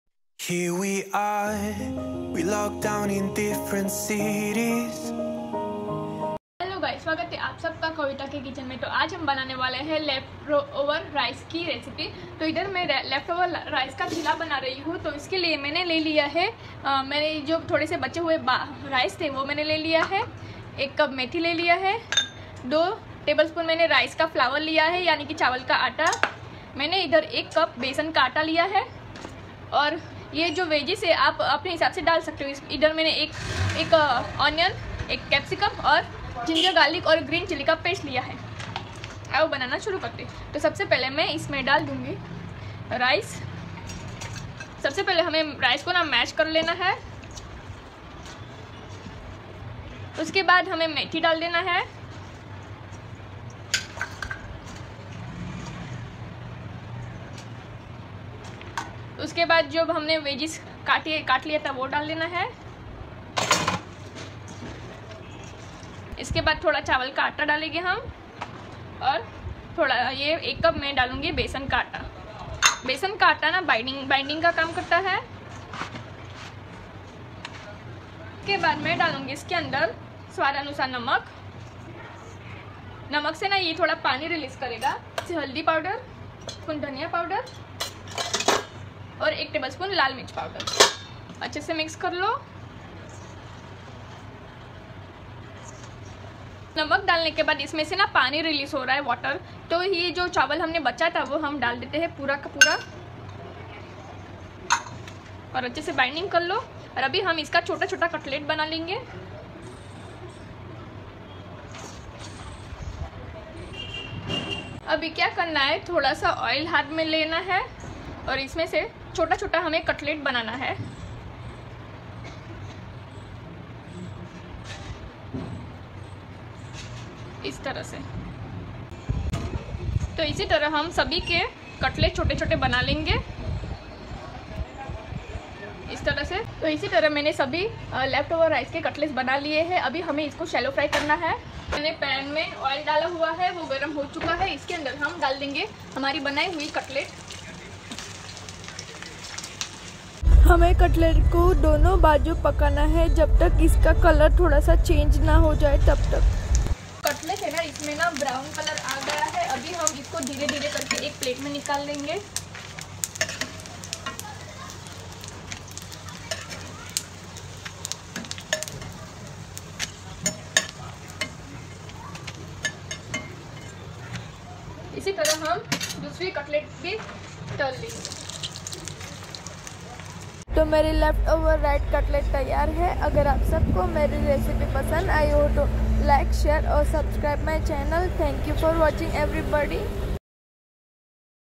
बचे हुए चावल से बनाए सुबह का नाश्ता/વધેલા ભાત ની રેસીપી/leftover rice recipe
2cup leftover rice
1cup methi
1cup besan
2 3 tsp rice flour
ginger garlic paste
1capcicum
1onion
Sal to taste
1tsp coriander powder
half tsp turmeric
1tsp red chilly powder

બચેલા ચોખા રેસીપી
बचे हुए चावल से बनाए सुबह का नाश्ता।
बचे हुए चावल से बनाओ कटलेस।